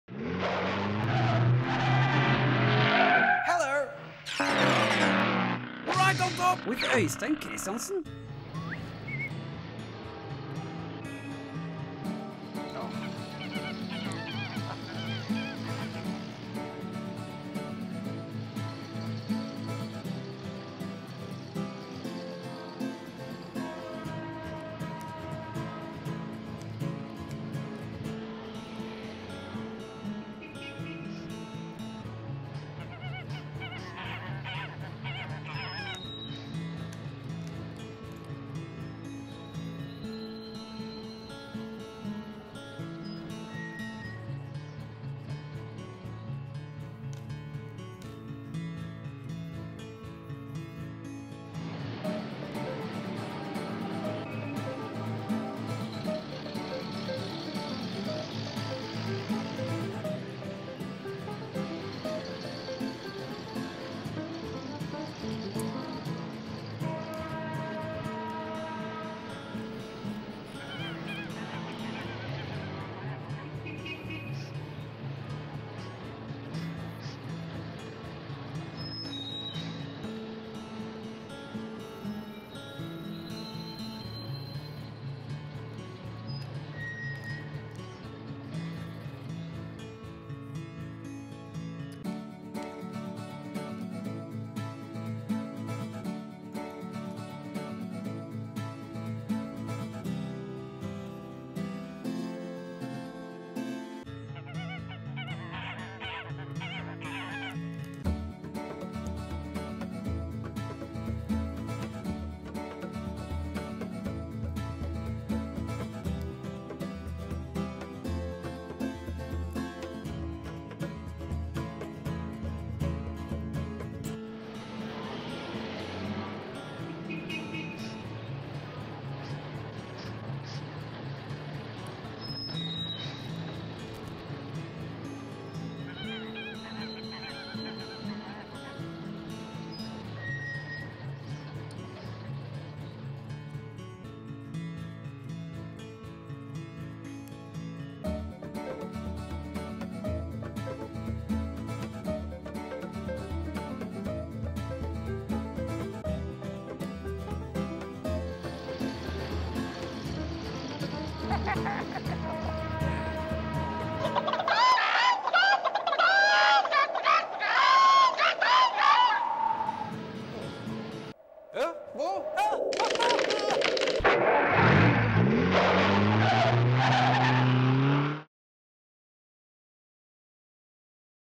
Crafts Ideas for Kids - Chicken | DIY on BoxYourSelf
In this episode of Right on Top Øistein makes a small easy craft, a 3D chicken, from ordinary objects you can find at home.

Every Tuesday and Friday we upload new DIY videos.